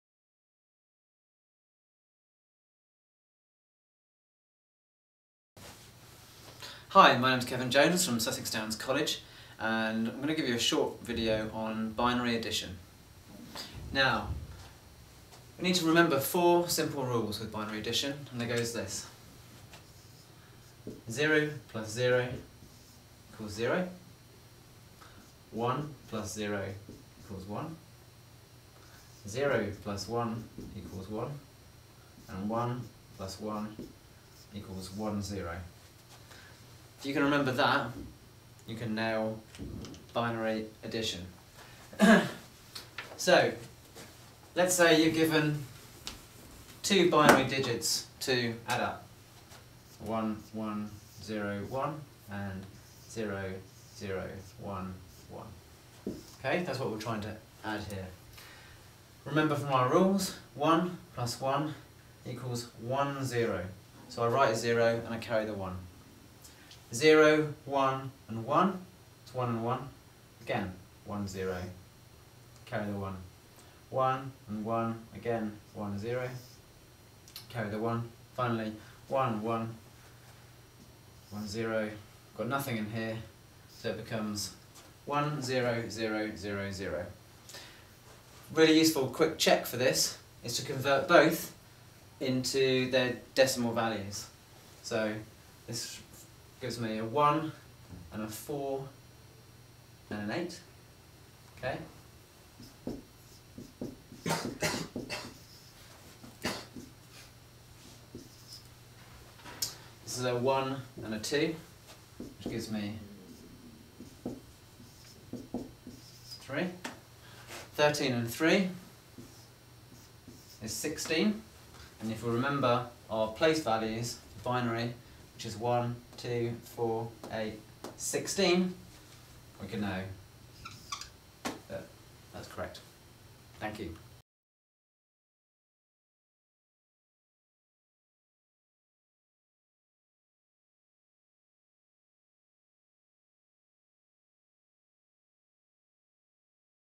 Binary Addition
Kevin Jones describes how to do Binary Addition